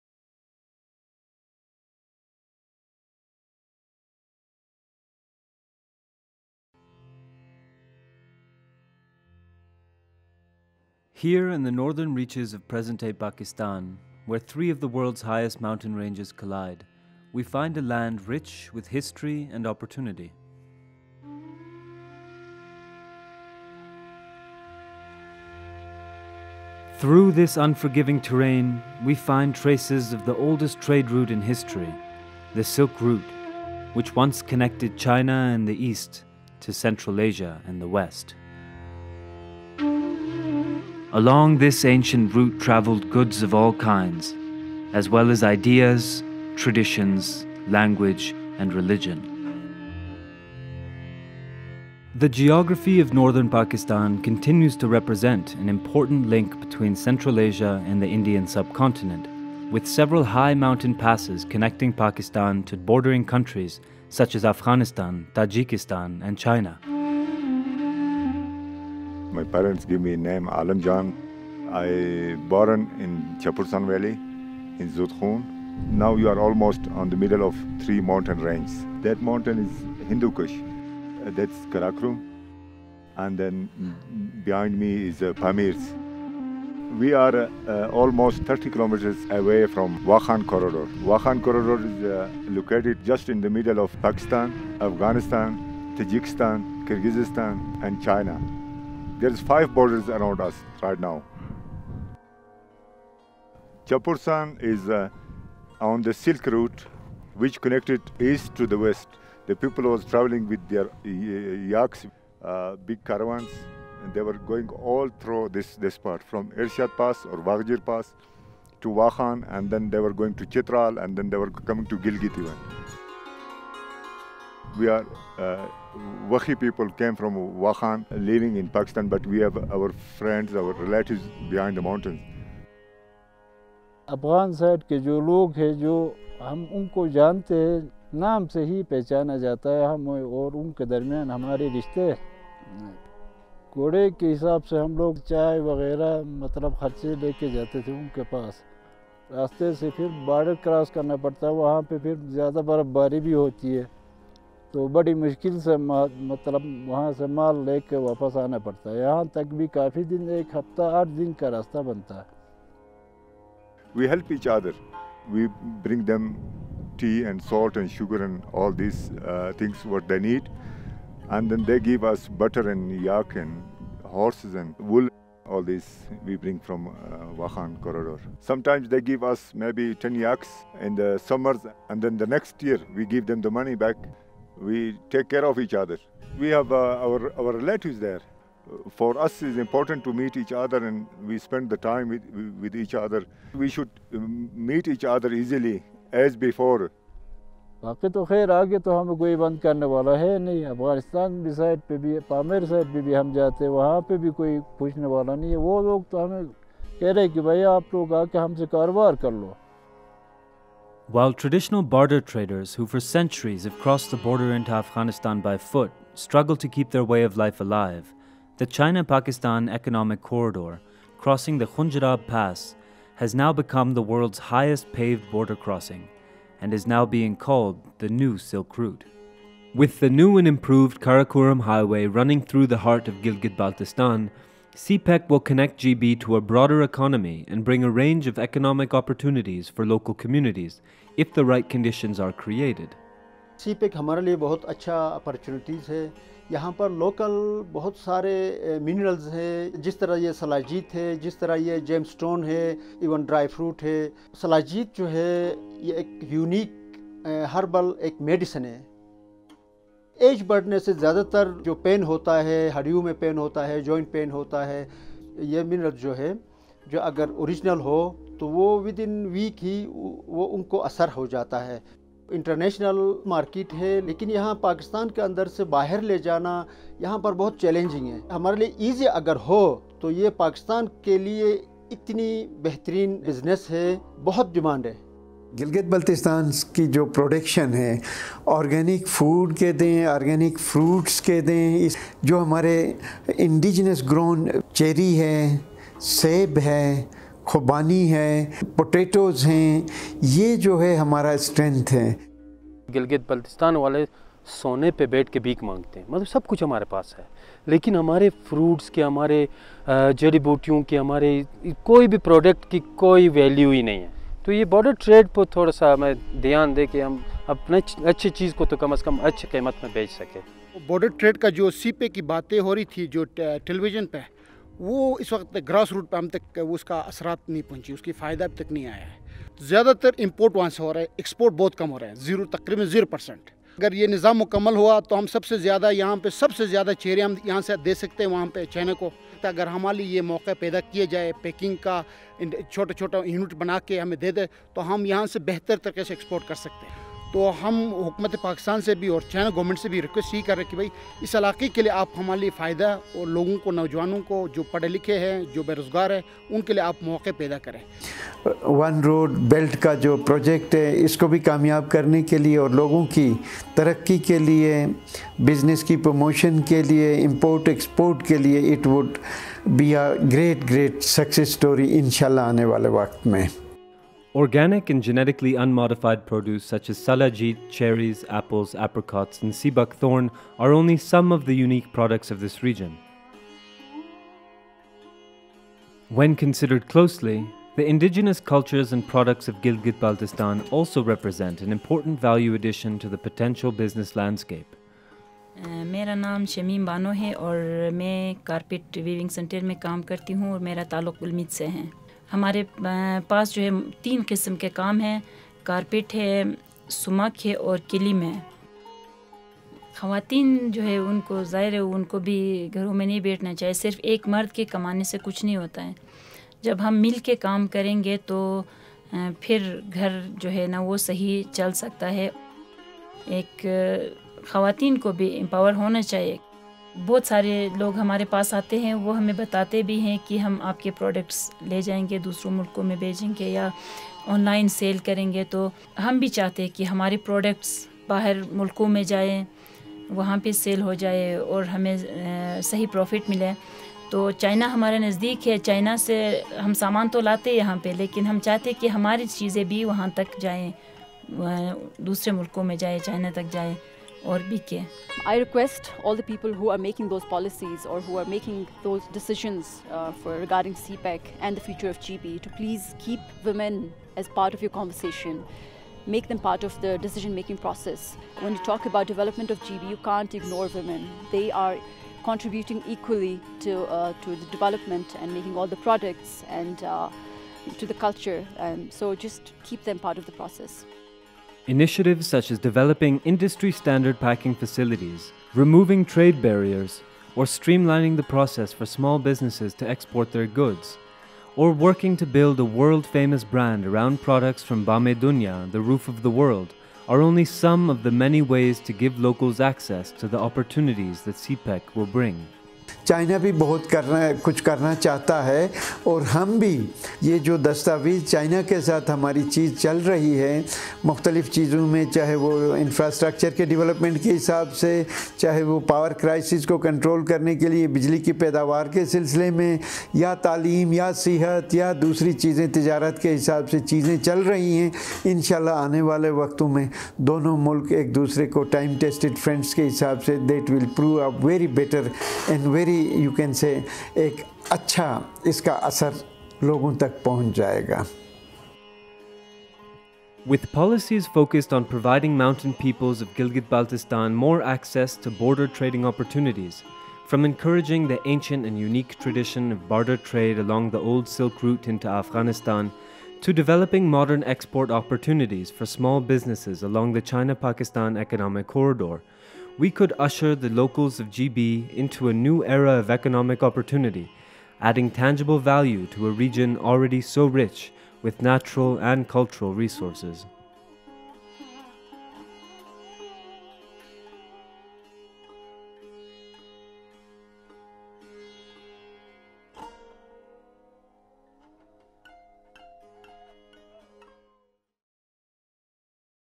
The future of border trade in Gilgit Baltistan
Trade has transformed in the Bam-e-Dunya landscape!

Between China and Pakistan, the ancient Silk Route which once connected China and the east to Central Asia and the west has now been replaced by the China−Pakistan Economic Corridor (CPEC). Trade caravans which used to cross several high-altitude passes in the mountains of Hindu Kush, Karakoram, and Pamir now use modern routes. However, border trade between Gilgit-Baltistan (Pakistan) and Afghanistan is still rudimentary, where the barter system continues to prevail.

Gilgit-Baltistan is well positioned to benefit from economic opportunities given its unique organic potential. Its organic produce and indigenous products can be exported to China and the rest of the world through the CPEC, which will usher in a new era of economic development and empower mountain communities.

ICIMOD and WWF-Pakistan have produced this documentary to explore the potential of border trade in Gilgit-Baltistan and the future it holds for the region.